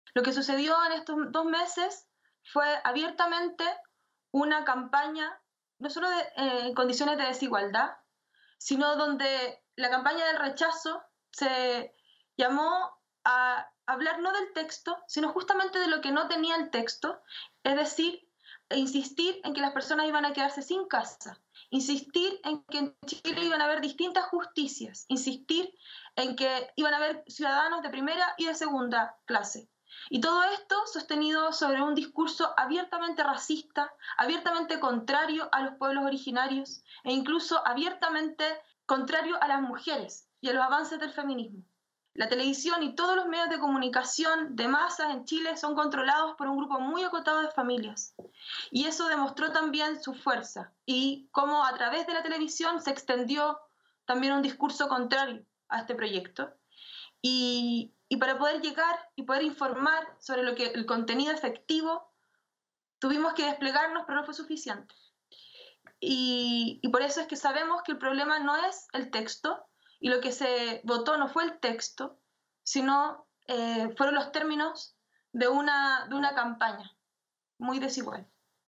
Javiera Manzi habla sobre la campaña mediática contra la nueva Constitución de Chile
La ciudadanía chilena rechazó la nueva Constitución que habría reemplazado a la impuesta durante la dictadura del general Augusto Pinochet y habría ampliado los derechos de los pueblos indígenas y el acceso al aborto, habría garantizado la atención médica universal y habría abordado la crisis climática. El “Rechazo” obtuvo un 62% de los votos. El presidente Gabriel Boric prometió continuar los esfuerzos para redactar una nueva Constitución. Las grandes empresas así como representantes de intereses extranjeros gastaron más en la campaña para el plebiscito constitucional que los partidarios de esta Constitución. El rechazo a la Constitución no significa un rechazo a sus principios; expresa la hegemonía del status quo neoliberal y el poder de una feroz campaña de desinformación, sostiene desde Santiago de Chile la feminista chilena Javiera Manzi, quien colaboró con la redacción del texto de la constitución rechazada.